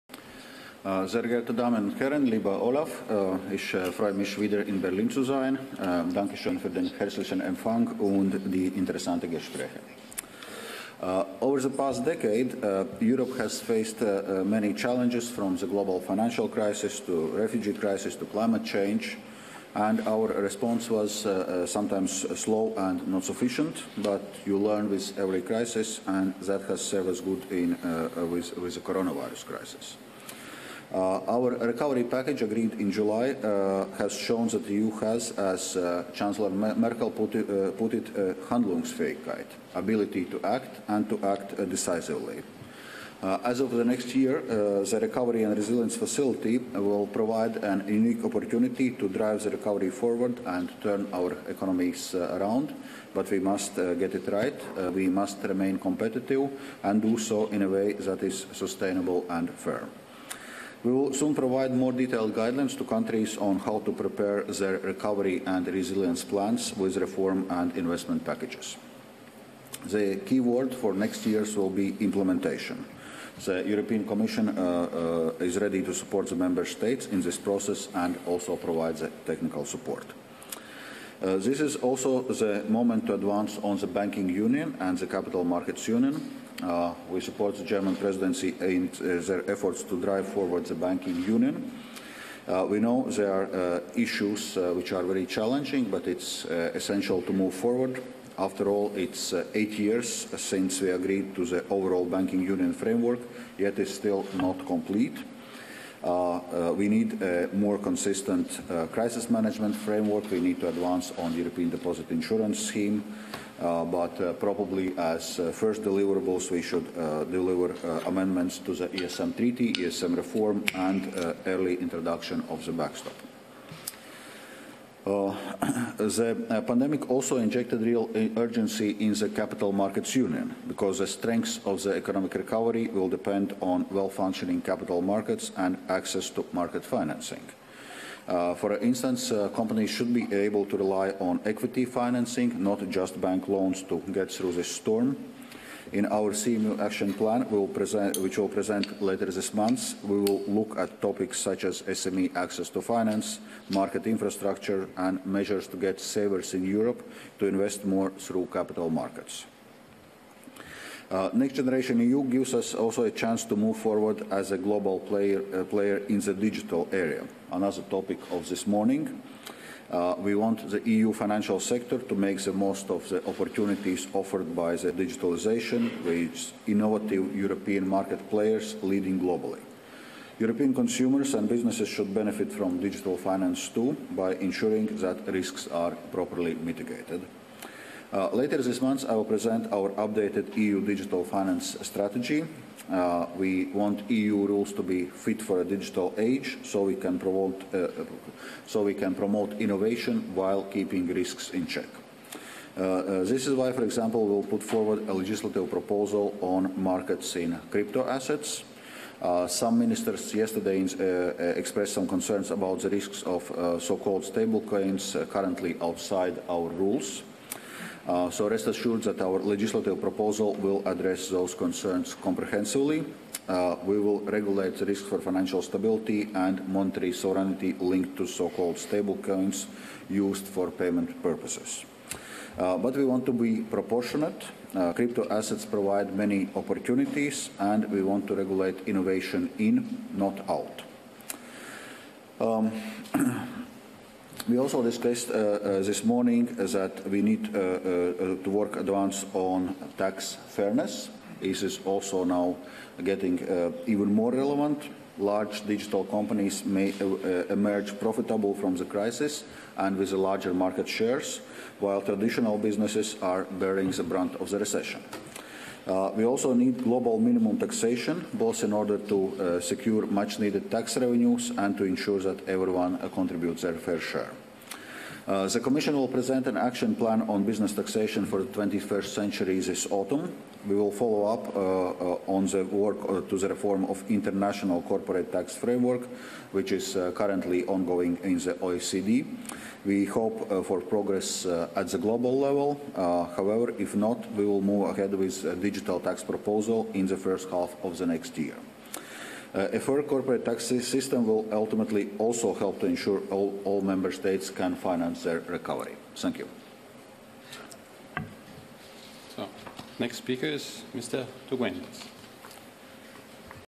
Dombrovskis asks for Capital Market Union, TAX fairness and Control of Digital Currenciews
Remarks by Executive Vice-President Dombrovskis at the informal ECOFIN press conference.

Ladies and Gentlemen, lieber Olaf,

Ich freue mich wieder in Berlin zu sein. Dankeschön für den herzlichen Empfang und die interessanten Gespräche.

Over the past decade, Europe has faced many challenges, from the global financial crisis, the refugee crisis, to climate change.

Our response was sometimes slow and insufficient. But you learn with every crisis – and that has served us in good stead with the Coronavirus.

The deal on our recovery package reached in July at the European Council meeting has shown that the EU has – as Chancellor Merkel put it – “Handlungsfähigkeit”: the ability to act – and to act decisively.

As of next year, the Recovery and Resilience Facility will provide a unique opportunity to drive the recovery forward and turn our economies around. We must get it right. We must remain competitive, and do so in a way that is sustainable and fair.

We will soon provide more detailed guidance to countries on how to prepare their Recovery and Resilience Plans with reform and investment packages.

The key word for the next years is IMPLEMENTATION. The European Commission stands ready to support the Member States at every step of the process and provide the technical support.

This is also the moment to complete the Banking Union and the Capital Markets Union.

We support the German Presidency in its efforts to drive forward the Banking Union. We know some issues are very challenging but it's essential we move forward. After all, it is 8 years since we agreed to the overall Banking Union framework, yet it still not complete.

We need a more consistent crisis management framework. And we need to advance on the European deposit insurance scheme but probably, as first deliverables, we should deliver amendments to the ESM Treaty, ESM reform and the early introduction of the backstop.

CAPITAL MARKETS UNION

We launched work on the Capital Markets Union before the crisis. But the pandemic has injected real urgency into the CMU - because the strength of economic recovery will depend on well-functioning capital markets and access to market financing.

For instance, companies should be able to rely on equity financing, not just bank loans, to get through this storm. In our CMU action plan we will present later this month, we will look at topics such as SME access to finance, market infrastructure, and measures to get savers in Europe to invest more through capital markets.

DIGITAL CURRENCIES

Next Generation EU gives also us the chance to move forward as a global player in the digital area – another topic of this morning. We want the EU financial sector to make the most of the opportunities offered by digitalisation, with innovative European market players leading globally. European consumers and businesses should benefit from digital finance too, while ensuring that risks are properly mitigated.

Later this month, I will present our updated EU Digital Finance Strategy.

We want EU rules to be fit for the digital age, so we can promote innovation while keeping risks in check. That is why, for example, we will put forward a legislative proposal on markets in crypto-assets.

Some ministers yesterday expressed some concerns about the risks of so-called stablecoins currently outside our rules. Rest assured that our legislative proposals will address those concerns comprehensively. We will regulate the risks for financial stability and monetary sovereignty linked to so-called ‘stablecoins' used for payments purposes.

But we want to be proportionate: crypto assets provide many opportunities, and we want to regulate innovation in, not out.

TAX FAIRNESS

As we discussed this morning, we also need to work on tax fairness. This is even more relevant now.

Large digital companies may emerge more profitable from the crisis and with a larger market share, while traditional businesses are bearing the brunt of the recession. We also need global minimum taxation, both in order to secure much needed tax revenues and to ensure that everyone contributes their fair share.

The Commission will present an Action Plan on business taxation for the 21st century this autumn. We will follow up on the work to the reform of the international corporate tax framework, which is currently ongoing in the OECD. We hope  for progress at the global level – and if not, we will move ahead with a digital tax proposal in the first half of next year.

A fair corporate tax system will ultimately help ensure all Member States can finance the recovery.